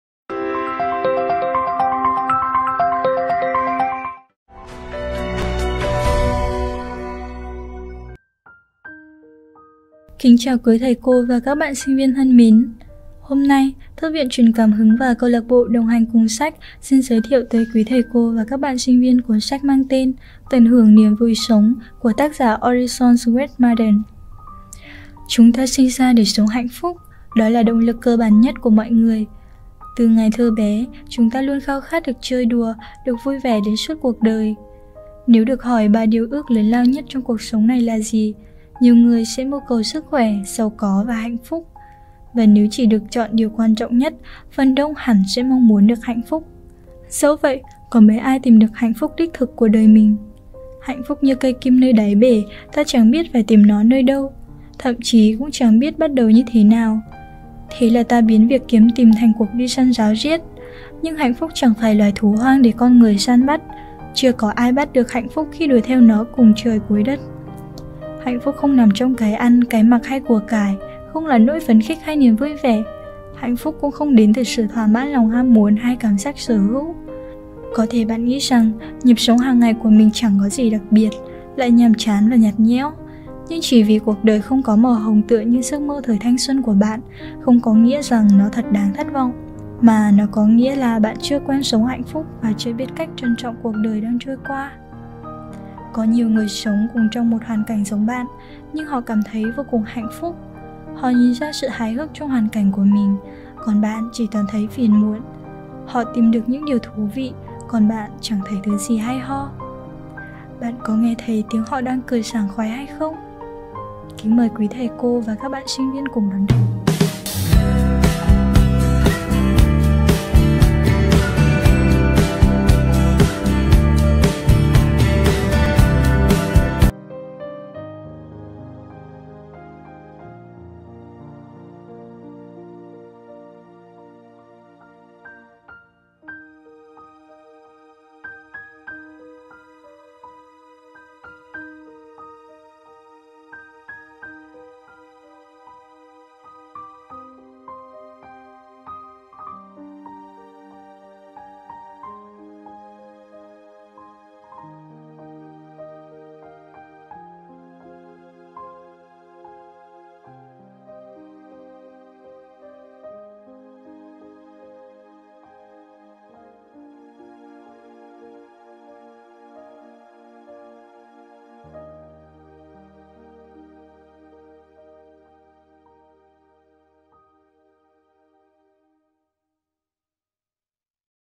Tận hưởng niềm vui sống | Orison Swett Marden
Chúng ta sinh ra để sống hạnh phúc. Đó là động lực cơ bản nhất của mọi người. Từ ngày thơ bé, chúng ta luôn khao khát được chơi đùa, được vui vẻ đến suốt cuộc đời. Nếu được hỏi ba điều ước lớn lao nhất trong cuộc sống này là gì, nhiều người sẽ mưu cầu sức khỏe, giàu có và hạnh phúc. Và nếu chỉ được chọn điều quan trọng nhất, phần đông hẳn sẽ mong muốn được hạnh phúc.
Có nhiều người sống cùng một hoàn cảnh giống bạn, nhưng họ cảm thấy vô cùng hạnh phúc. Họ nhìn ra sự hài hước trong hoàn cảnh của mình, còn bạn chỉ toàn thấy phiền muộn. Họ tìm được những điều thú vị, còn bạn chẳng thấy thứ gì hay ho. Bạn có nghe thấy tiếng họ đang cười sảng khoái hay không?